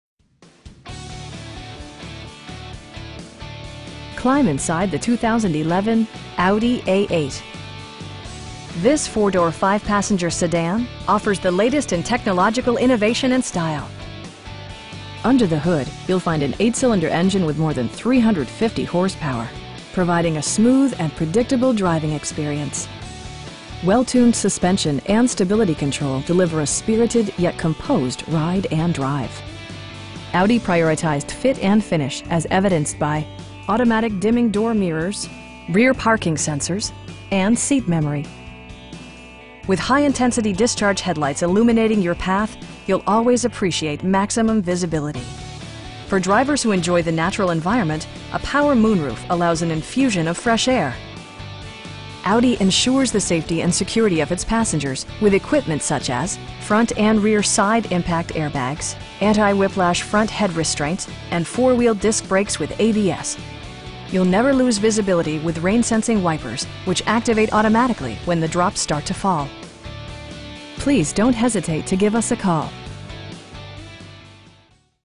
2011 Audi A8 L in Riverside, CA 92504
Walter's Audi
3210 Adams Street

Climb inside the 2011 Audi A8. This 4 door, 5 passenger sedan offers the latest in technological innovation and style. Under the hood you'll find an 8 cylinder engine with more than 350 horsepower, providing a smooth and predictable driving experience. Well tuned suspension and stability control deliver a spirited, yet composed, ride and drive Audi prioritized fit and finish as evidenced by: 1-touch window functionality, a built-in garage door transmitter, a trip computer, automatic dimming door mirrors, rear parking sensors, turn signal indicator mirrors, and seat memory. Everything is where it ought to be, from the dashboard controls to the door locks and window controls. With high intensity discharge headlights illuminating your path, you'll always appreciate maximum visibility. For drivers who enjoy the natural environment, a power moonroof allows an infusion of fresh air. Audi ensures the safety and security of its passengers with equipment such as: dual front impact airbags, front and rear SIDE impact airbags, traction control, brake assist, anti-whiplash front head restraints, a panic alarm, and 4 wheel disc brakes with ABS. You'll never lose visibility with rain sensing wipers, which activate automatically when the drops start to fall. We pride ourselves in the quality that we offer on all of our vehicles. Please don't hesitate to give us a call.